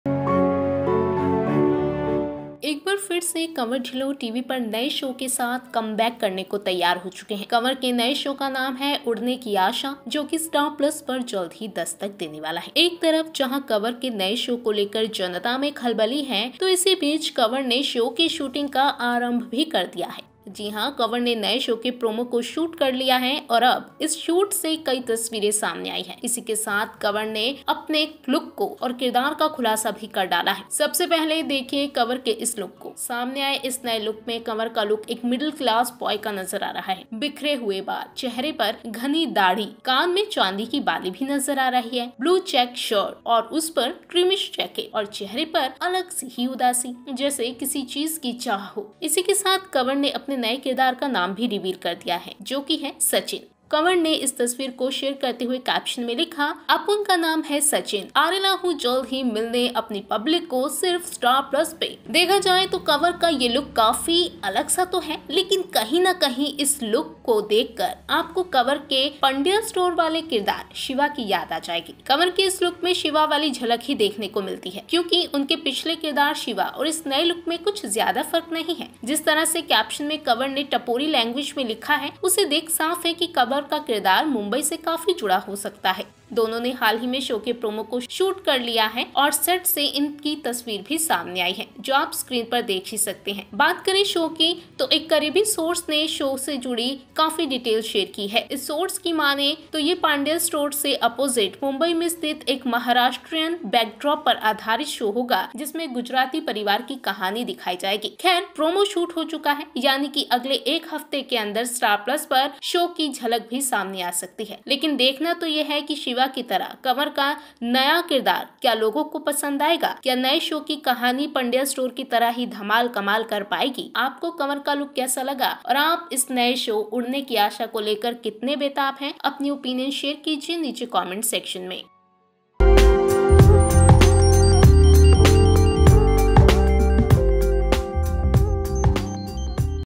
Kanwar Dhillon Starts Shooting His New Show; Here’s 1st Look !
Kanwar Dhillon Shares His 1st Look From His New Show Udne Ki Asha!